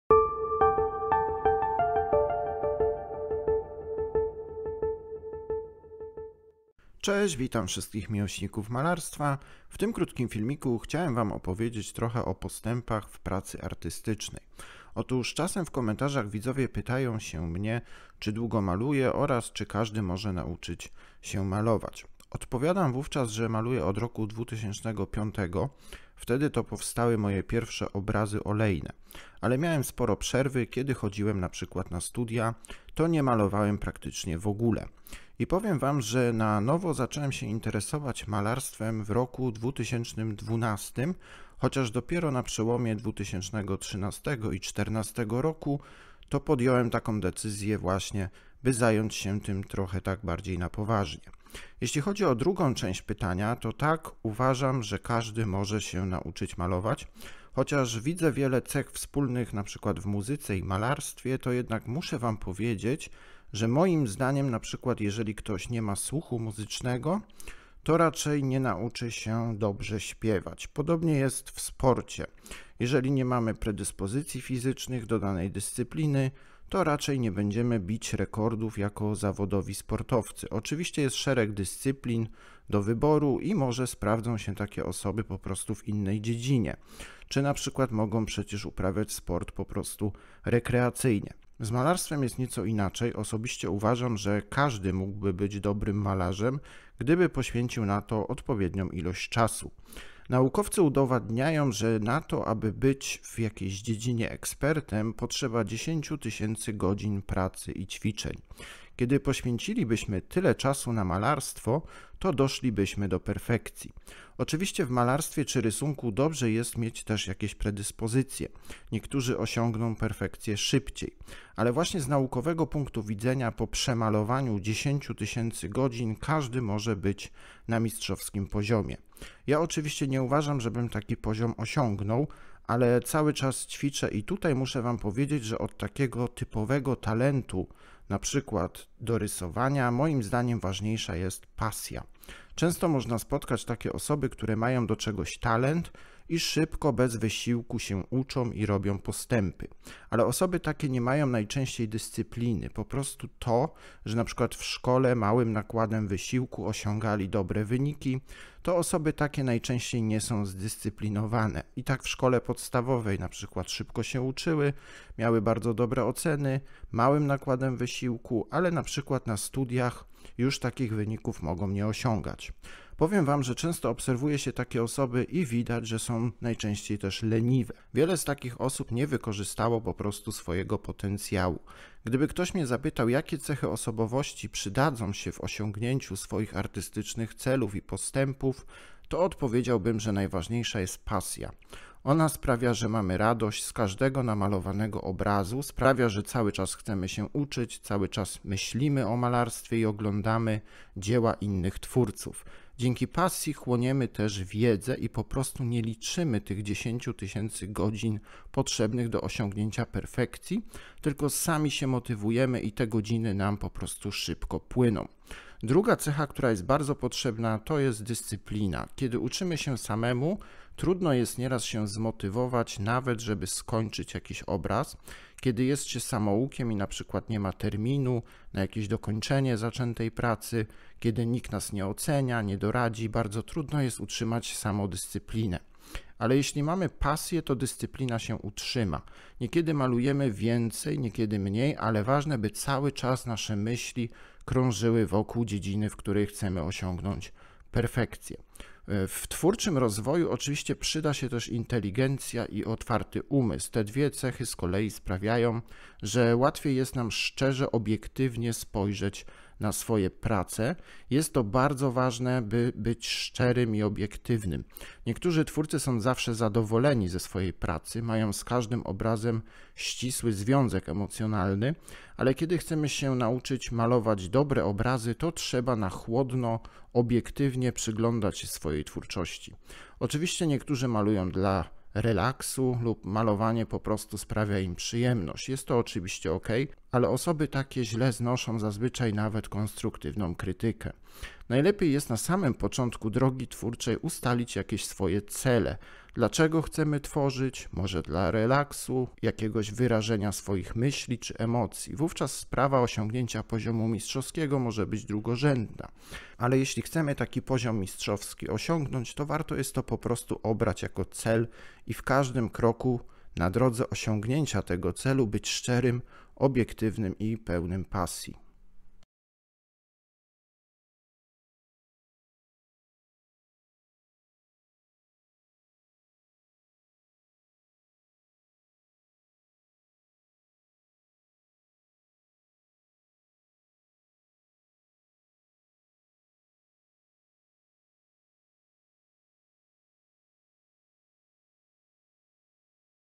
Co warto wziąć pod uwagę, gdy ze swoją twórczością chce się wejść na wyższy poziom
Witam wszystkich miłośników malarstwa. W tym krótkim filmiku pragnę podzielić się swoimi przemyśleniami na temat twórczego rozwoju.